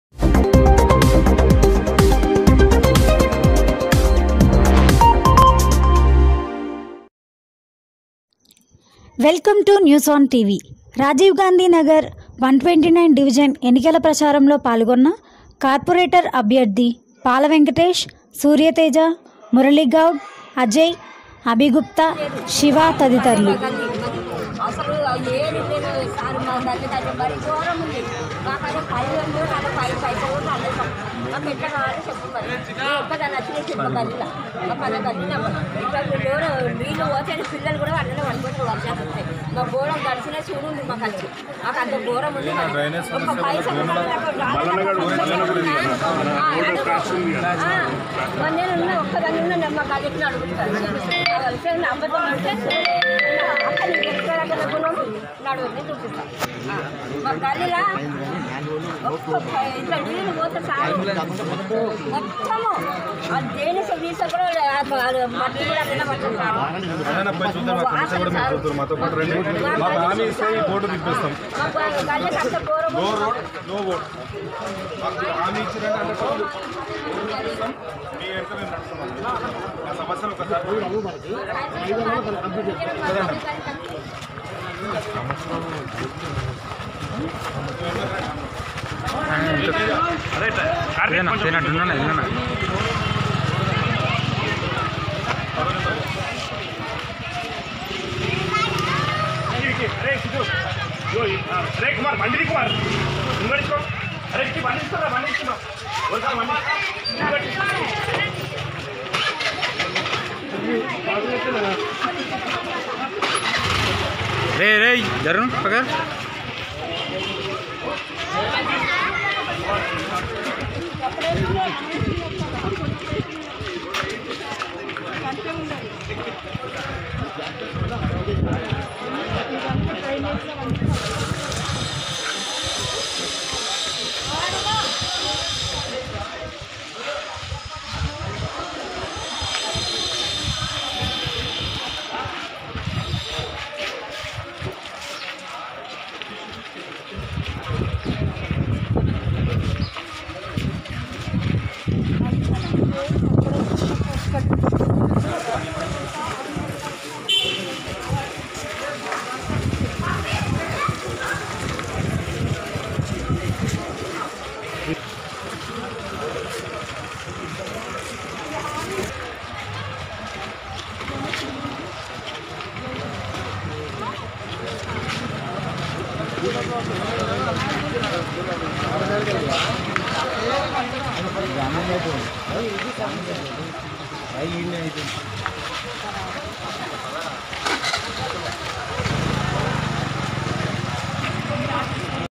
129 డివిజన్ రాజీవ్ గాంధీ నగర్ లో కొర్పొరేటర్ అభ్యర్థి పాల వెంకటేష్ ఎన్నికల ప్రచారం | NEWS ON TV
129 డివిజన్ రాజీవ్ గాంధీ నగర్ లో  కొర్పొరేటర్ అభ్యర్థి పాల వెంకటేష్ ఎన్నికల ప్రచారం  | NEWS ON TV